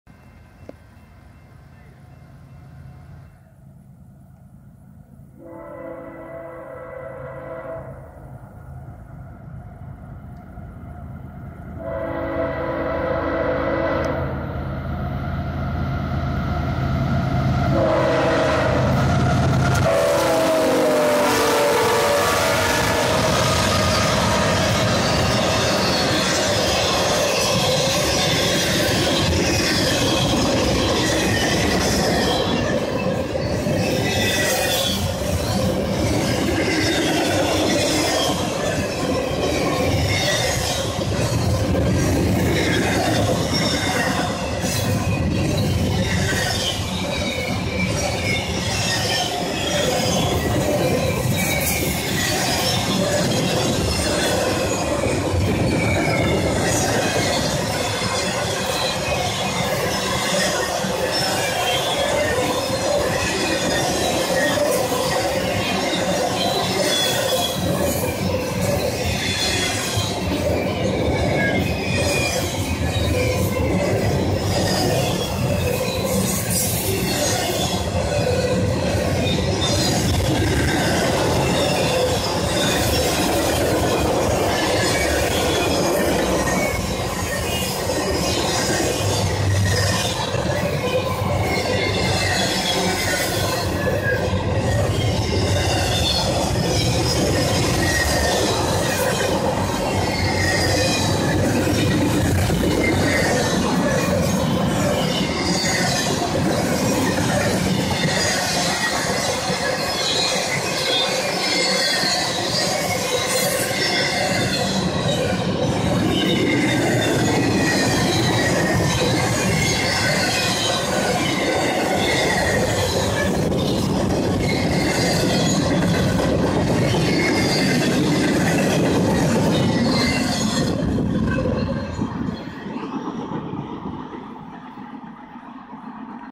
[HD] CSX Q162 Intermodal Train Eastbound - Fairport NY
{Q162}

[CSXT] GE CW44AC #262
[CSXT] GE ET44AH #3324

©️2017UpstateNewYorkRailfan